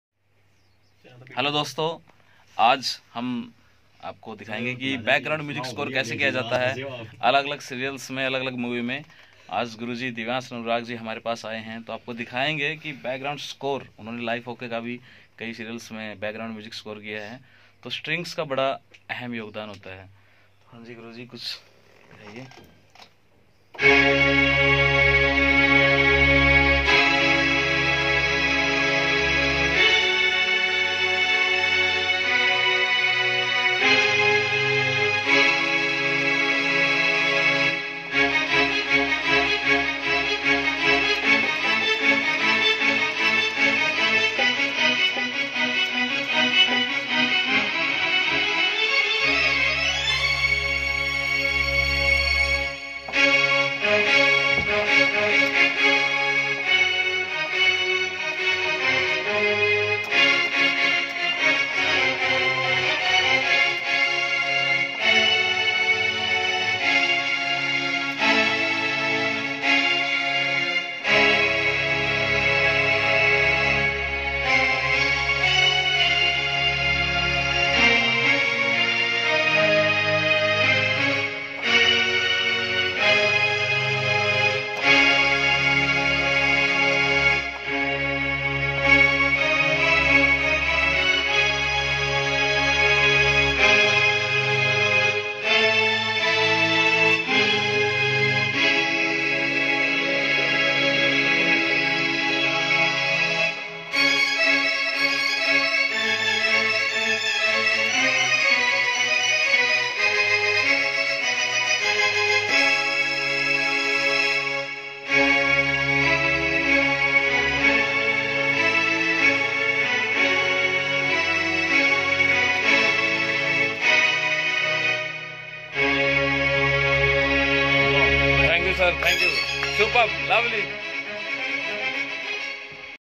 लाइव Background म्यूजिक स्कोर देखें in Movie|| Background Music Score live By Sh. Divyansh Anurag Ji
आइए लाइव देखिए कैसे बैकग्राउंड म्यूजिक स्कोर किया जाता है Movies एंड सीरियल्स में